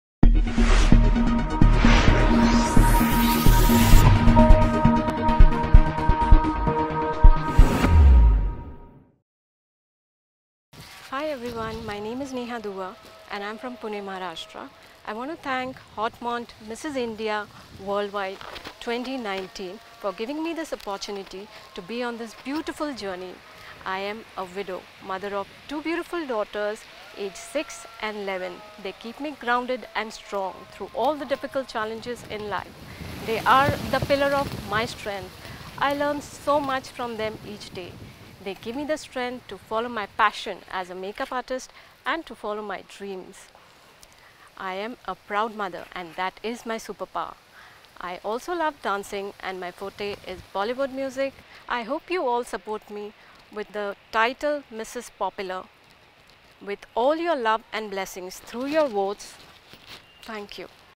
Neha Dua, Finalist, Haut Monde, Mrs. India Worldwide 2019
Neha Dua gets the strength to follow her dreams through her daughters! Her superpower is being a mother!